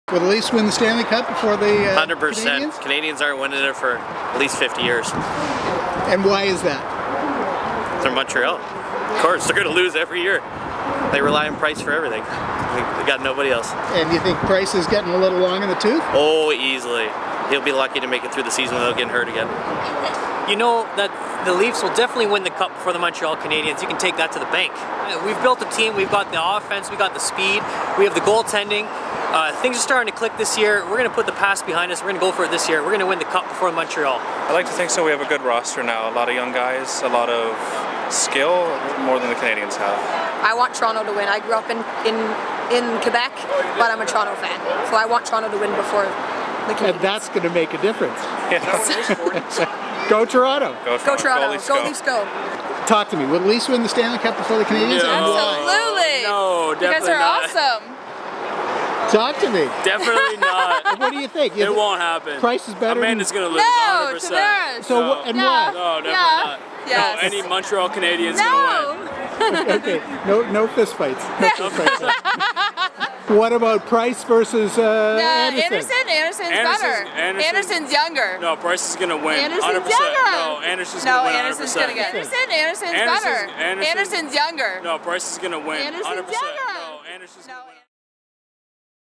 SUN STREETER: Will the Leafs win the Stanley Cup before the Canadiens?
Hockey fans were asked prior to tonight's game between the Leafs and the Habs,  as to the which team would win the Stanley Cup first?